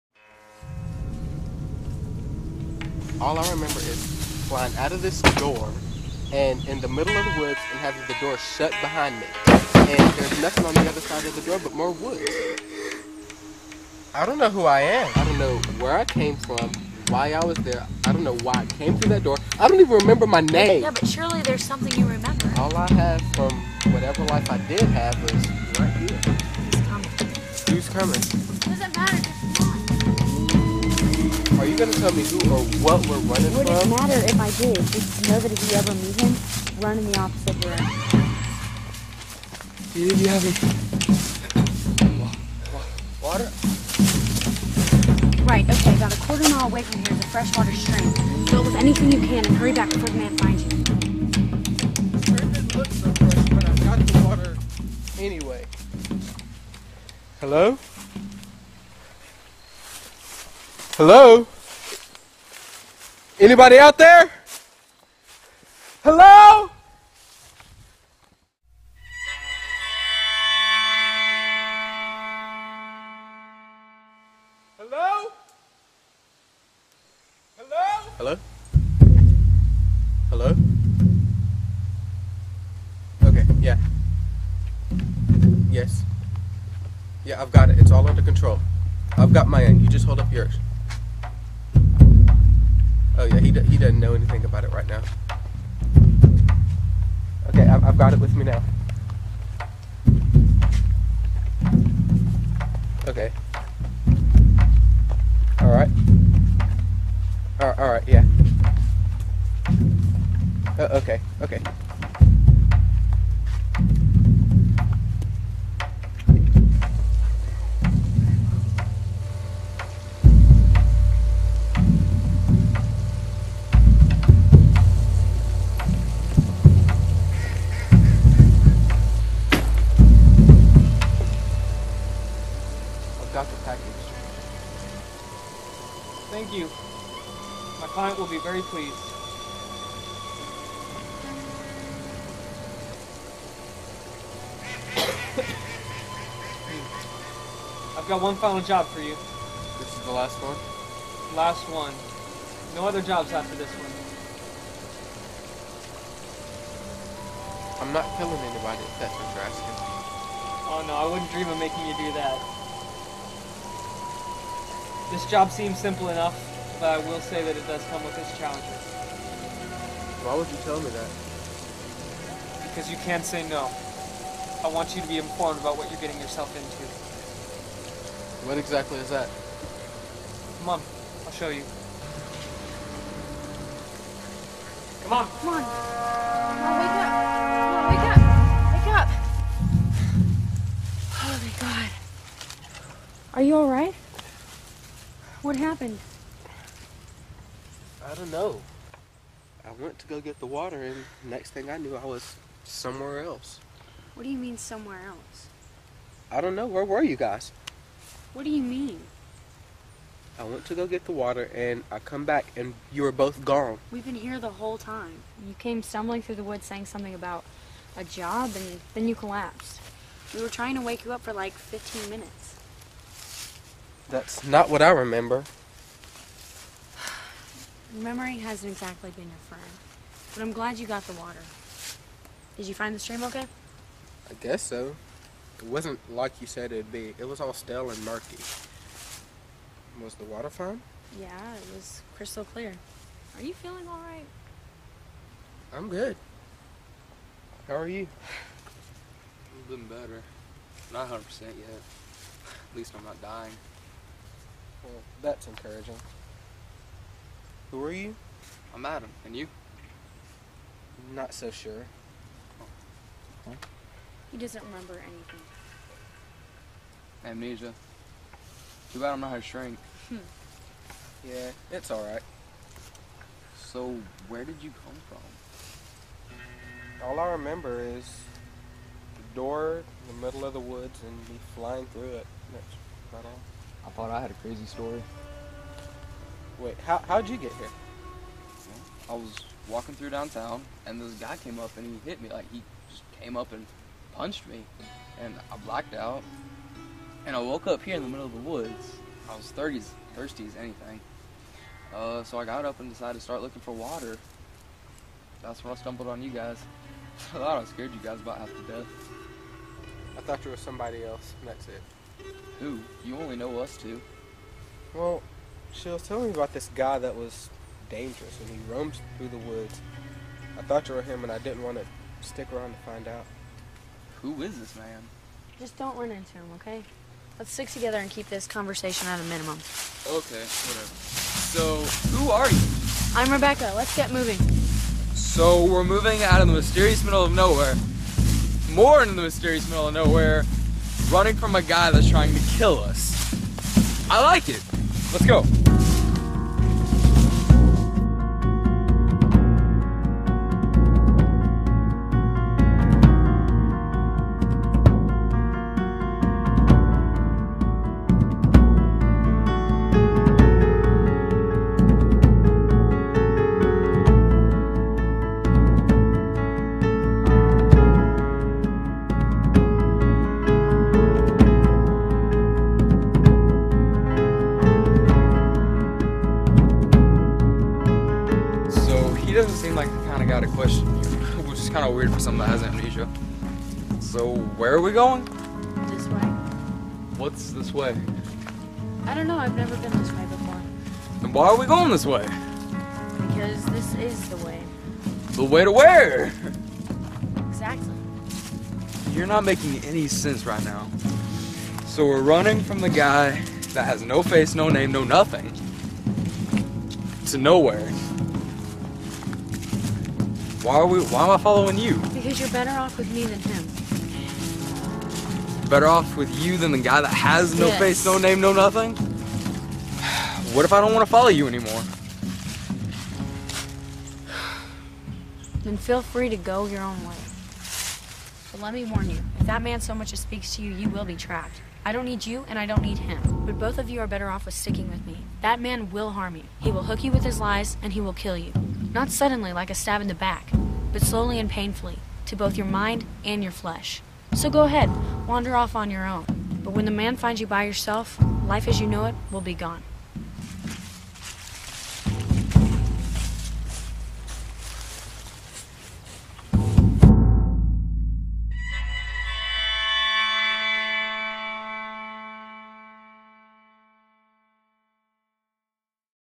The Door: "The New Man"
The man with no name returns to his camp to find it vacant of the two people who were supposed to be there.  Another dream begins to haunt him as more mysteries are thrust upon him.